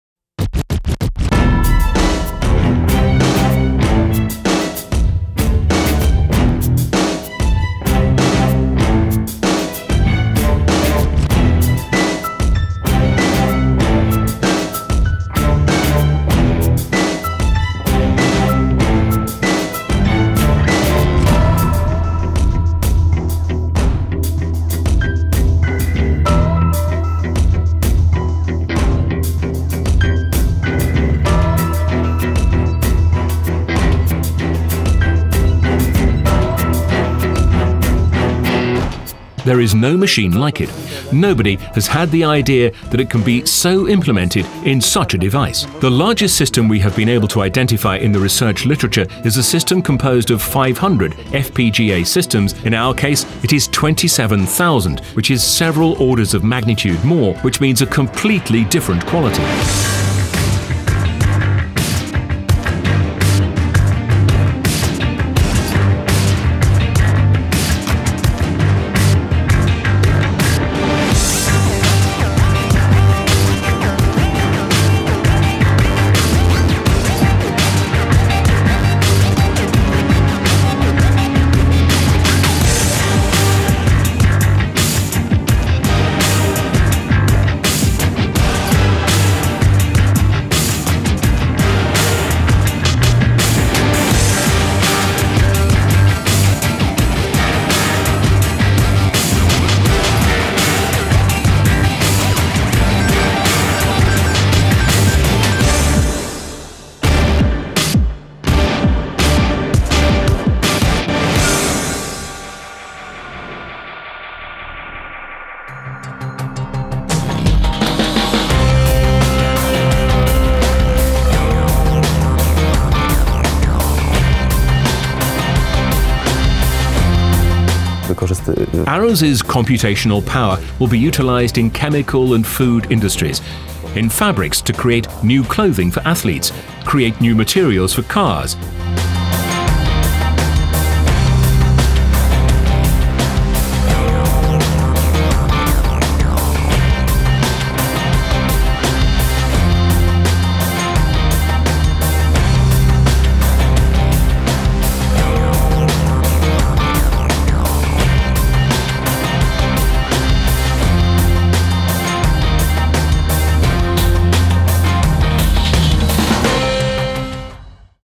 ARUZ
Największy na świecie symulator procesów fizykochemicznych.
Tysiąc razy szybszy od obecnych superkomputerów.
30 tysięcy układów FPGA, 75 tys kabli, 100 km połączeń elektrycznych, 14 miesięcy budowy.
Zbudowany w łódzkim Technoparku przez Ericpol, według koncepcji stworzonej przez pracowników Politechniki Łódzkiej.
Analizator Rzeczywistych Układów Złożonych.